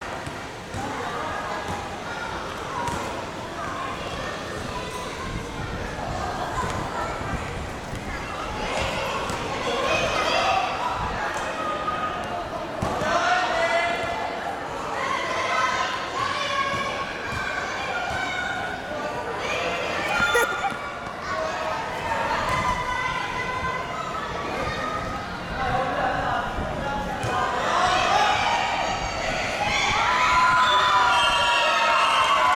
力國積極的搶球, 終於獲得回報, 1:1踢平
105年內湖春季理事長杯, 第一場